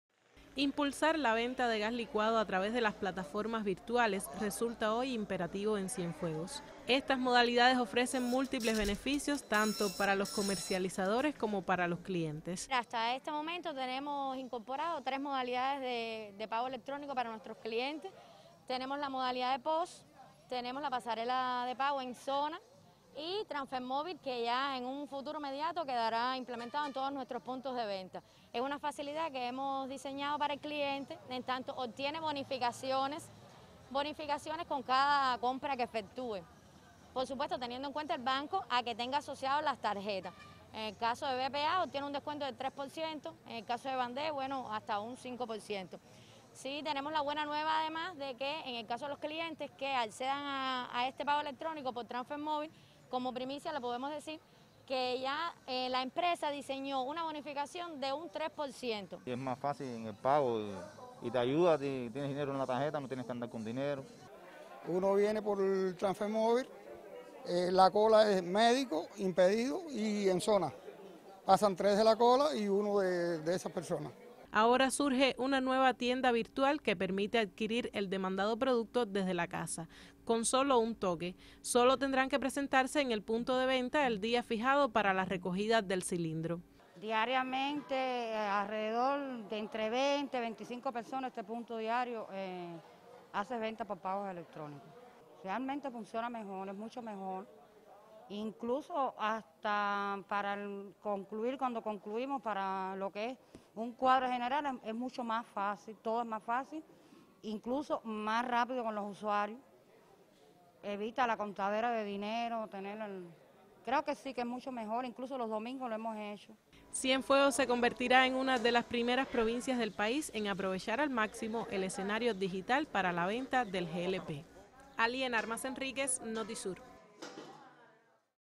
Amplía Cupet posibilidades en el pago electrónico
La Unidad Empresarial de Base División Territorial Comercialización de Combustibles Cienfuegos fomenta el comercio electrónico. Plataformas como Transfermóvil y Enzona se suman a la creación de una nueva tienda virtual.